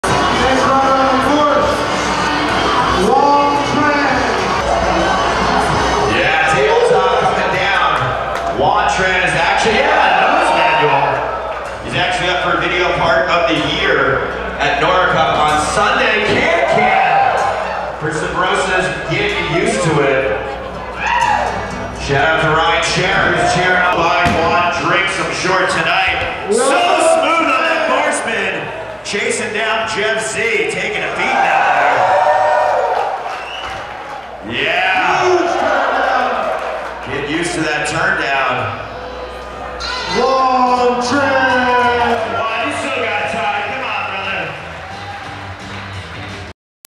Hoang Tran at Texas Toast
Take a quick look at some of Subrosa Brand pro rider Hoang Tran's highlights from the Street course at the 2013 Texas Toast Jam.